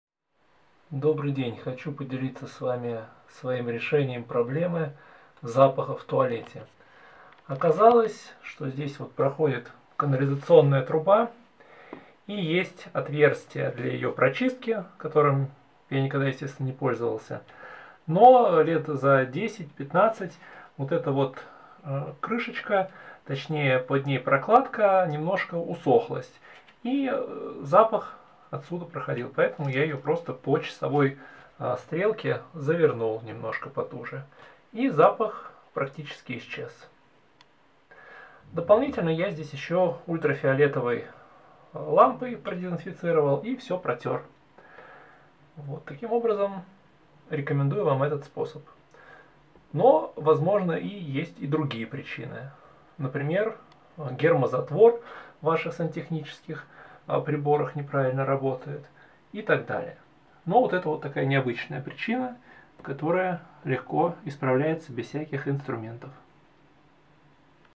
Как избавиться от запаха в туалете
Для того чтобы избавиться от запаха в туалете подкрутите по часовой стрелке крышку на отверстии для прочистки канализации.